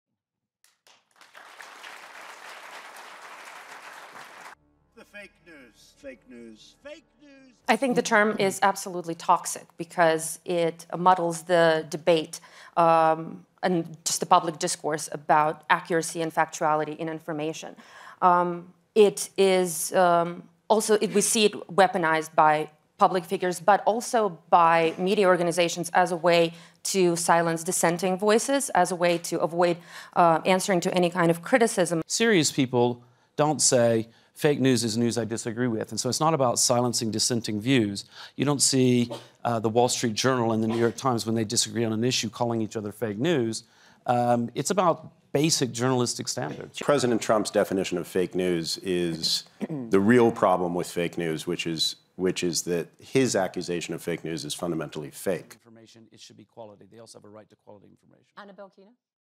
'Fake news' panel held at WEF in Davos
BBC journalist Zeinab Badawi hosts panel on fake news with Jimmy Wales, founder of Wikipedia, Joseph Kahn, managing editor of the New York Times, Anna Belkina, deputy chief editor of RT, and Bilawal Bhutto Zardari, chairman of the Pakistan People’s Party and son of assassinated prime minister Benazir Bhutto.